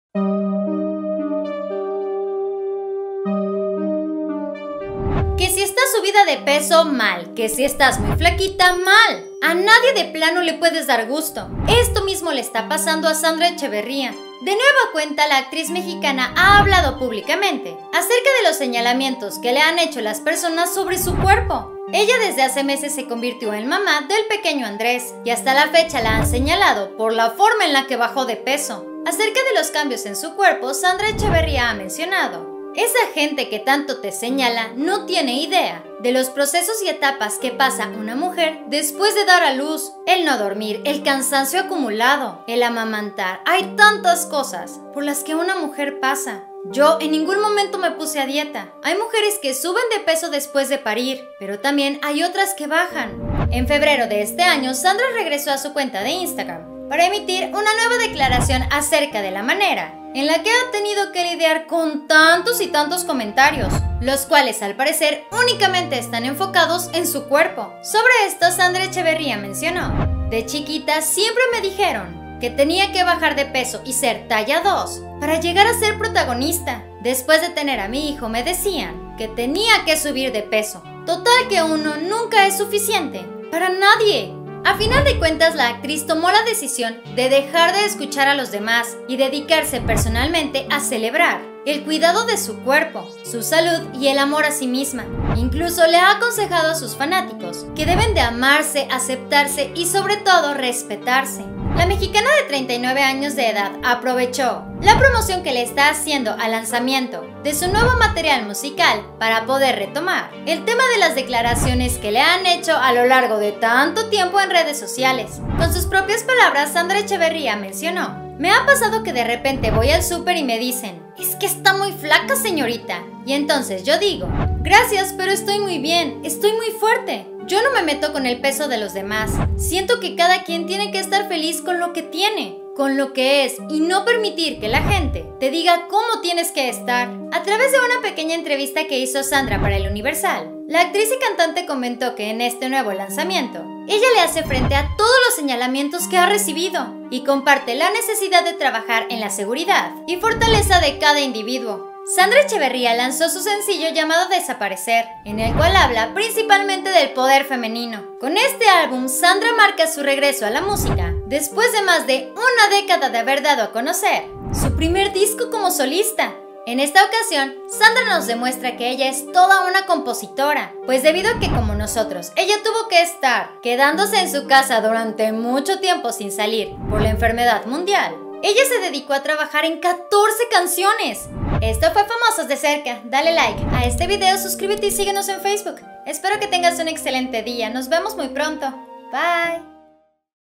⛔¡ HACE UNOS MOMENTOS ! ⛔ ¡ Sandra Echeverria CONFIESA Como Esta Su Salud HOY ! 🙏🙏
Sandra Echeverria rompe el silencio y nos cuenta su verdadera situacion de salud.

 BIENVENIDOS!!!